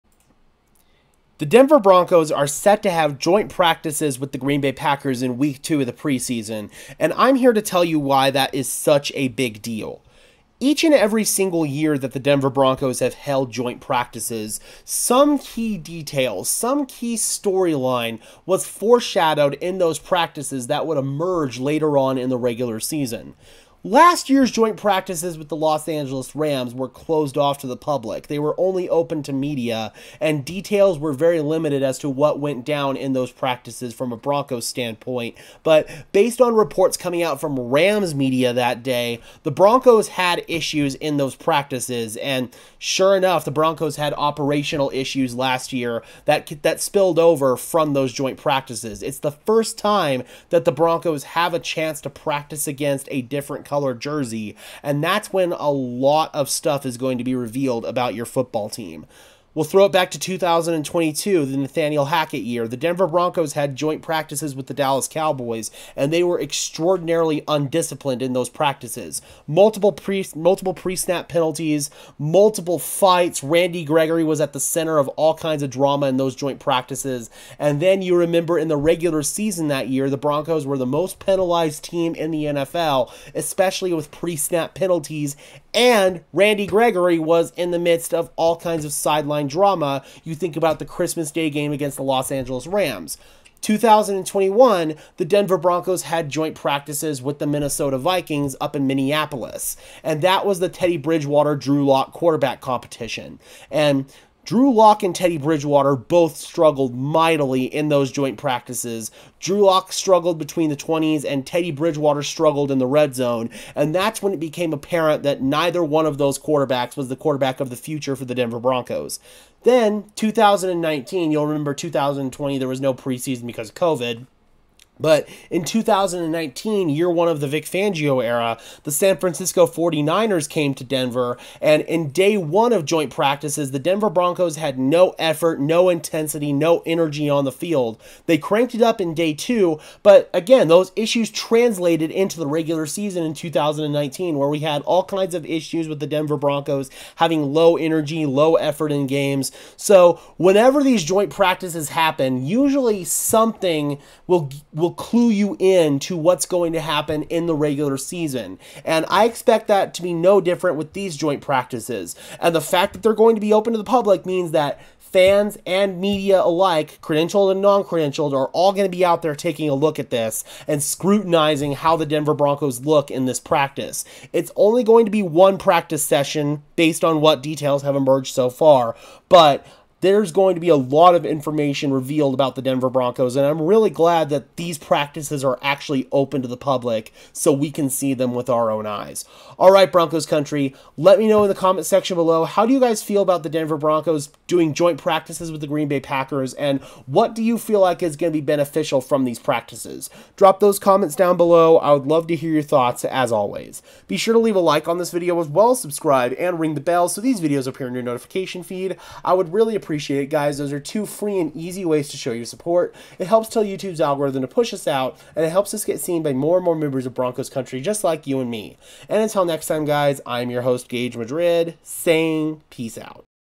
Denver Broncos JOINT PRACTICES with Green Bay Packers will Reveal KEY DETAILS about 2024 Season!!
Join us as we dive into the exciting joint practices between the Denver Broncos and the Green Bay Packers! In this video, we'll break down how these sessions are more than just warm-ups—they're critical for assessing player performance, testing new strategies, and understanding the team dynamics for the upcoming 2024 NFL season. Get insider insights on standout players, key matchups, and what these practices reveal about each team's readiness. Whether you're a Broncos or Packers fan, or just an NFL enthusiast, this analysis will give you a deeper understanding of what to expect when the season kicks off. Don't miss out on this exclusive look at the future of these two iconic franchises!
Broncos Syndicate Hosted by Gage Madrid